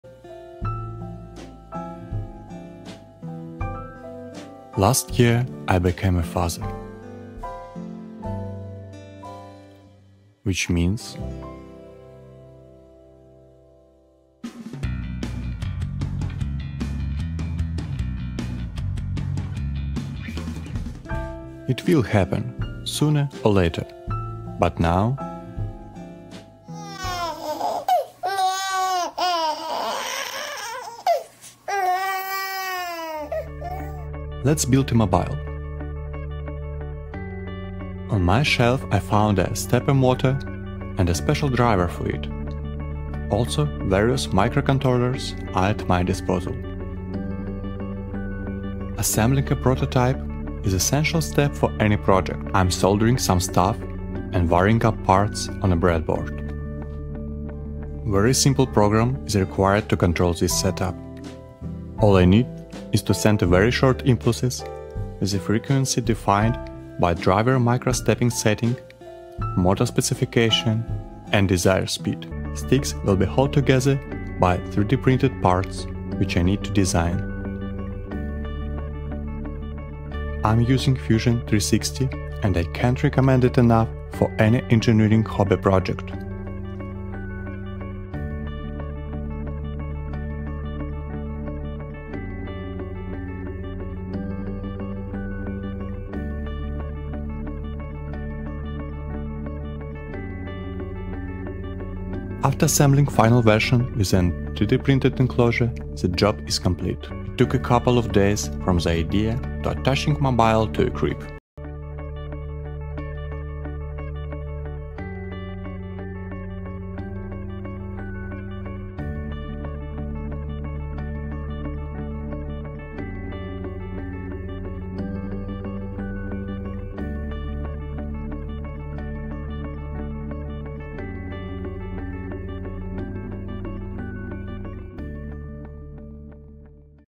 I Overengineered a Crib Mobile for My Newborn!
What do you get when a new dad with an engineering obsession builds a baby mobile? 🤖 A stepper-motor-powered, Arduino-controlled, 3D-printed masterpiece that could probably launch satellites. Okay, maybe not — but it does rock the crib.

In this project, Pavel Korshunov shows how he brought together a NEMA17# stepper motor, Arduino Nano, Fusion 360, carbon fiber rods, and pure dad energy to create the ultimate crib mobile. Is it overkill? Absolutely. Is it awesome? You bet.

Perfect mix of DIY, IoT, and dad jokes. 💡👶

Tools & Materials:
- NEMA17 steeper motor
- TMC2209 Stepper motor driver
- Noctua Premium Quiet Fan to keep driver cool
- Arduino (Elegoo) UNO board during prototyping
- Arduino NANO in a final product
- Step-down DC/DC voltage converter to use 12 volt for motor and 5v for logic board
- 608zz bearings
- 8 mm Carbon Fiber tubes
- 5/16" OD Vinyl-Flex PVC tubing
- Creality Ender 3 3D printer